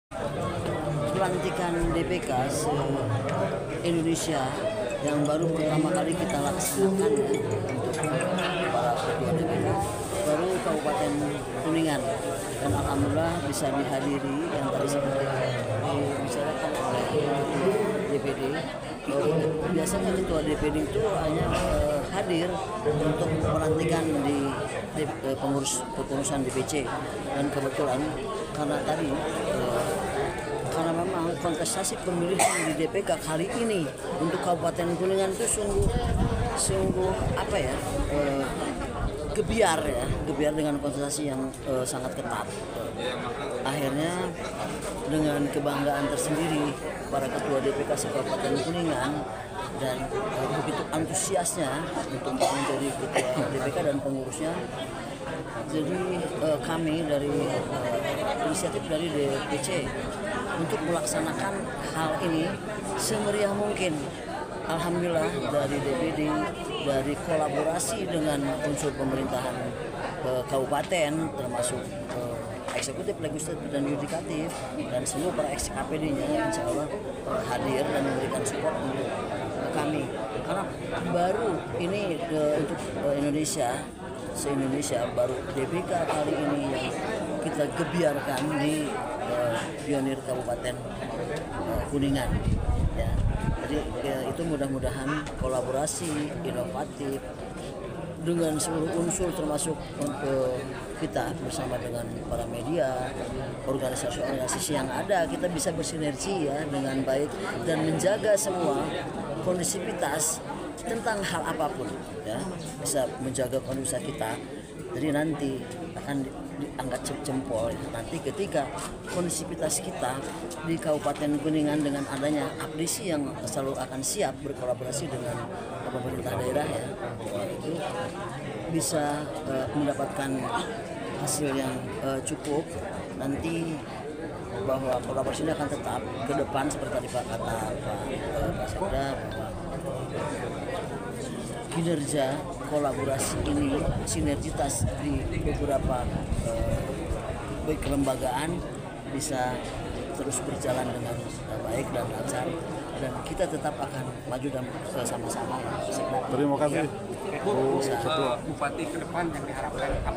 Hj. Henny Rosdiana, SH,S. Sos, M.Si Ketua APDESI Kab. Kuningan Lantik 32 DPK Kecamatan
Video dari GLOBAL RISE TV